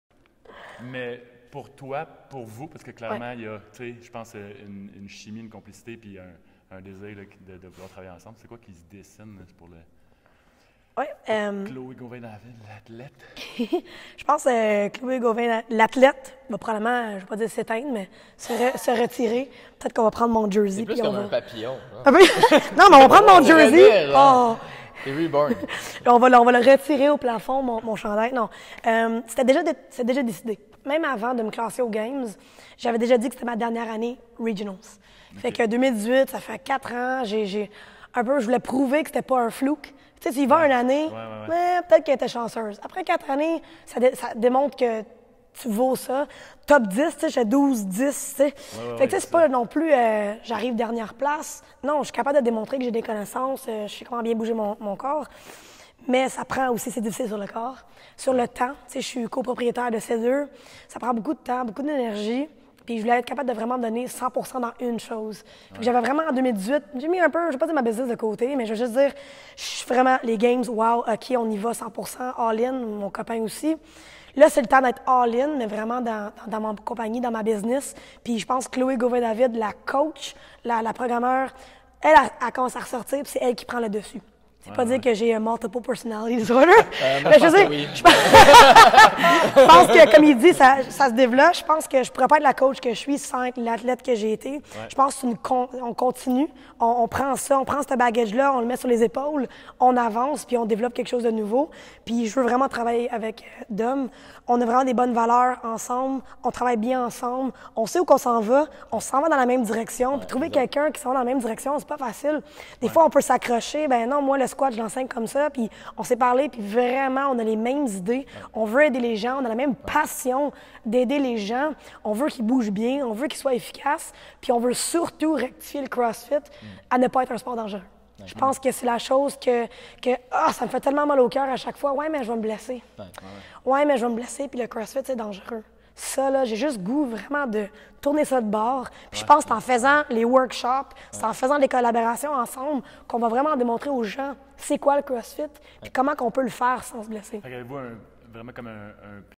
Entrevue avec CHLOÉ GAUVIN DAVID & DOMINIC BAILLARGEON - Partie 6: Post-Athlète
Entrevue exclusive avec Chloé Gauvin David et Dominic Baillargeon de Nxt Generation Physio et de Nxt G Mobili-T. Cette entrevue est divisée en plusieurs parties distinctes et couvrira de nombreux sujets concernant la carrière de Chloé et de Dominic dans le domaine du crossfit. Bon visionnement!